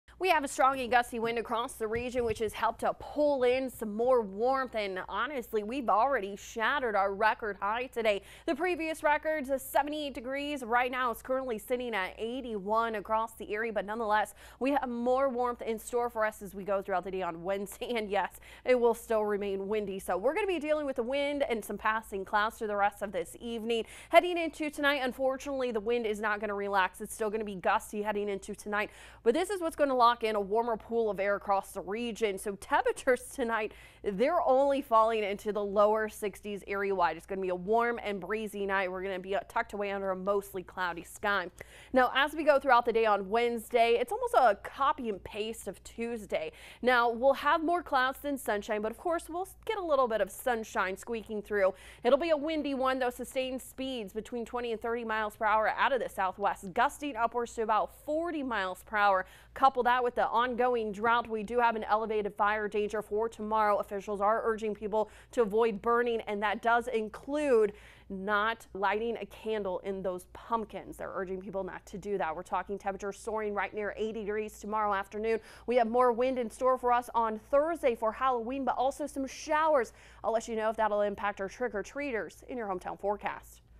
Record warmth Wednesday, elevated fire danger
Wednesday is a warm and windy day with highs topping out near 80 degrees.